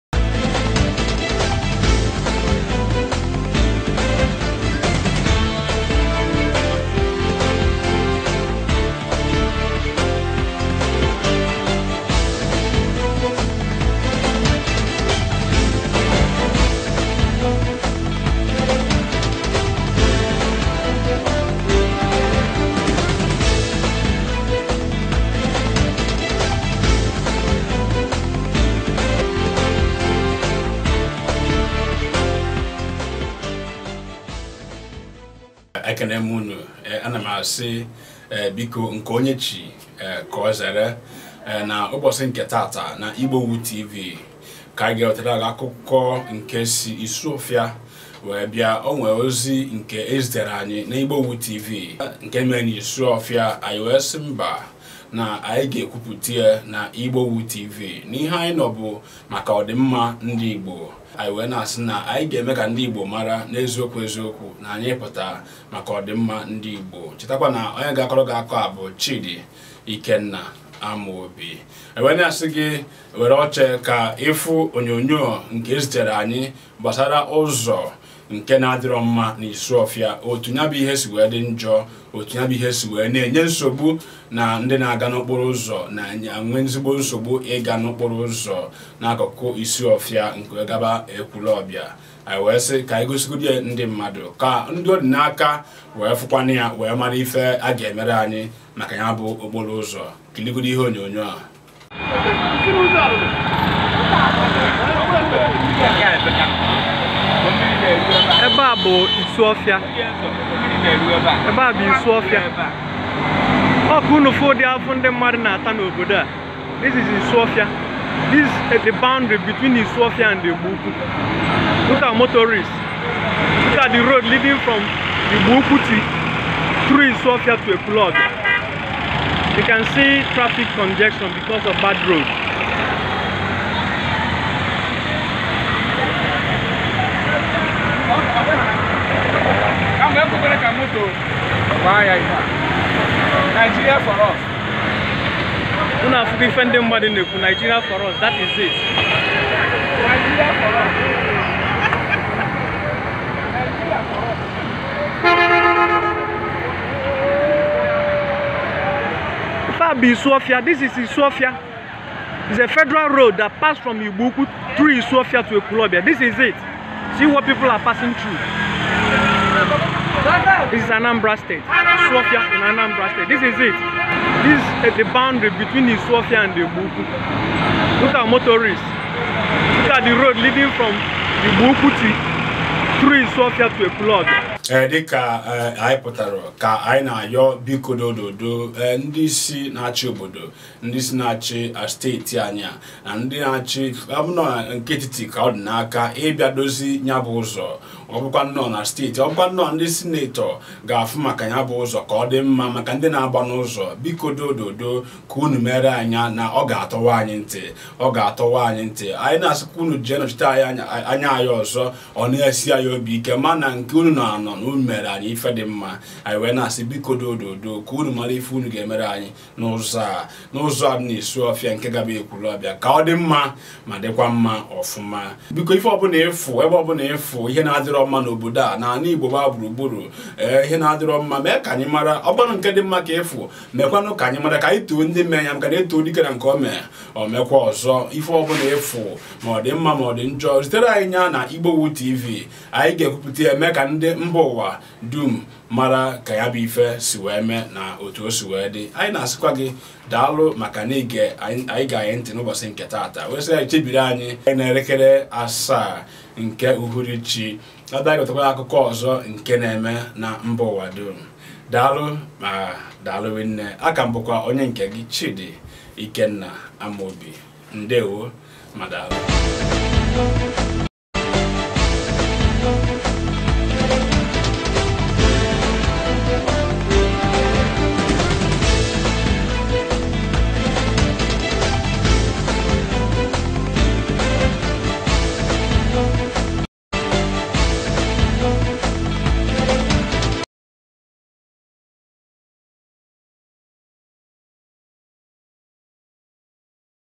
OKPORO UZO AGHOROLA OBA MMIRI NKE NIGER NA ANAMBRA STEETI...Igbowood tv
Ndi na aga na Okporo uzo na ebe akwa maka na ya bu uzo aburula ajo uzo puru iche.